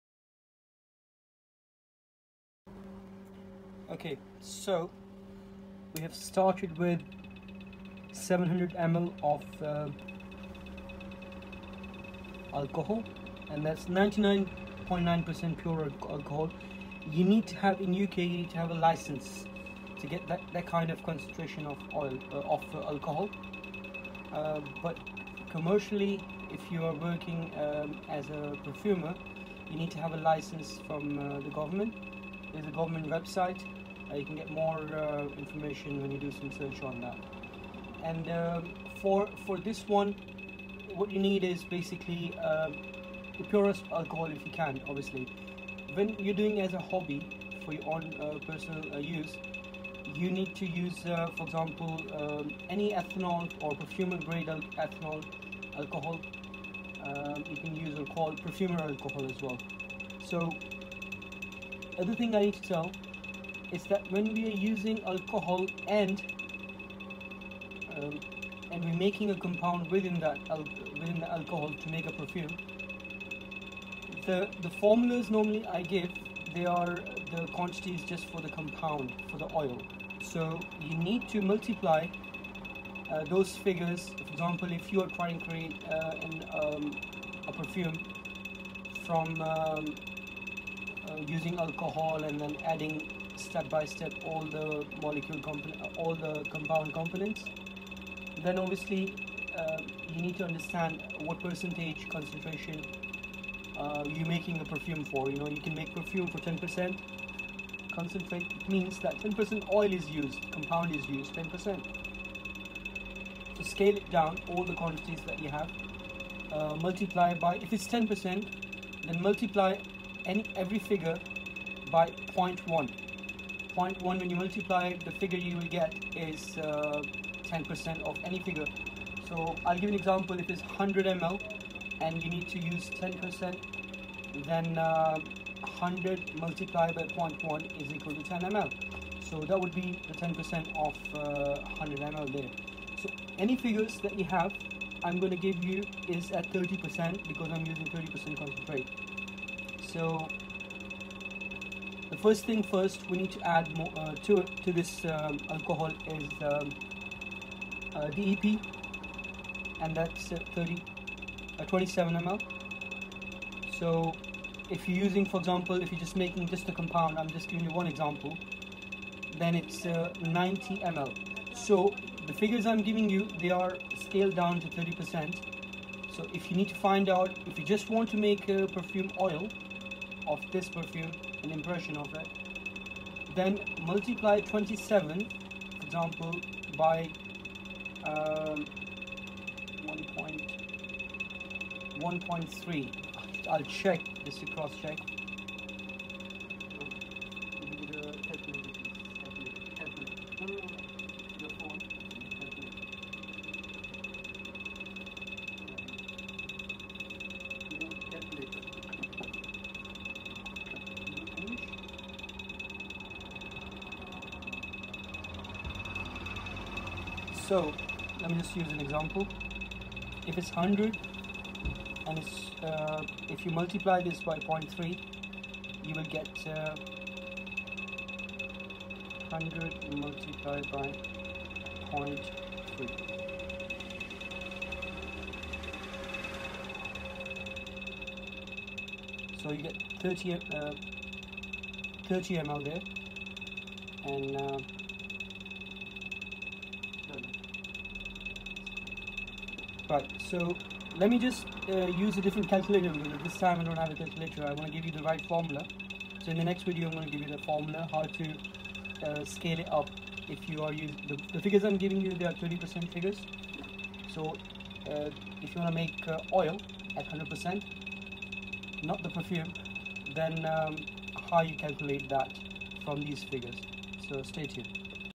Part 2 DIY MAKE JOOP Femme - perfume compound FOR WOMEN -ittar or attar -complete formula
0:00 INTRO JOOP FOR WOMEN
0:48 ANALYTICS OF NOTES
4:47 ISO E SUPER, LABDANUM, ETHNOL, DPG,
9:03 CUMARIN
10:35 GLAXOLIDE
11:52 CIS3 SUBSTITUTE
13:11 DIHYDROMERCENOL
14:21 CITRONELLOL
15:20 MUSK T OR ETHYLENE BRACELET
17:00 BERGAMOT
Part 4 Making Gucci ENVY for Women-
IN THIS VIDEO - WE DEMONSTRATE THE PROCESS OF LUXURY FRENCH PERFUME AND OIL  MAKING - THERE ARE TWO KINDS OF PRODUCTS YOU CAN MAKE THE PERFUME OIL ATTAR OR ITTAR & PERFUME SPRAY COLOGNE.
YOU CAN BUY THESE PRE MADE BASES, OILS AND PERFUMES  FROM OUR WEBSITE SOON
I also show you my preferred way to make a spray on perfume!